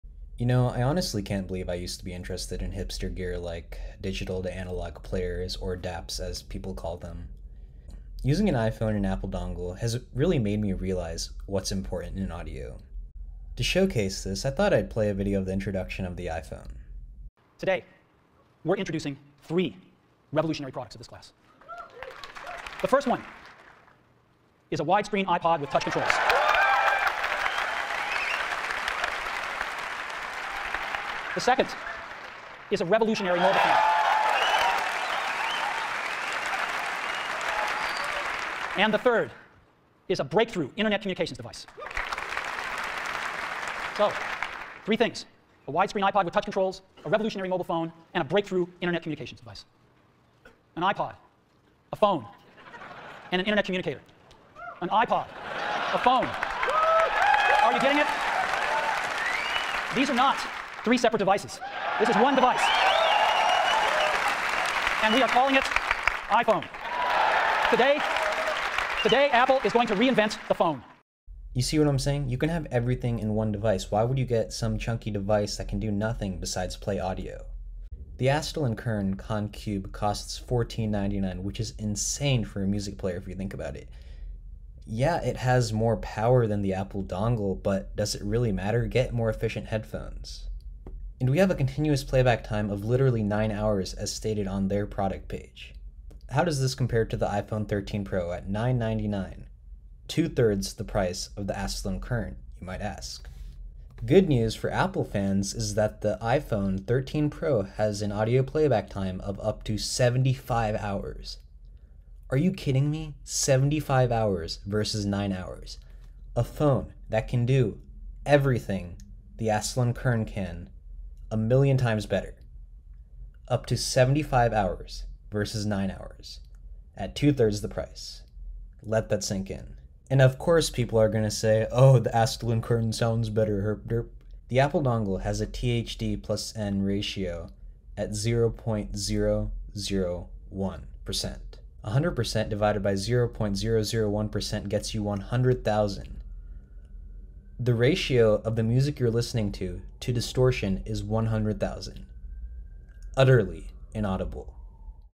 DAPS ARE FOR IDIOTS
Don't buy a dedicated audio player. Buy a phone.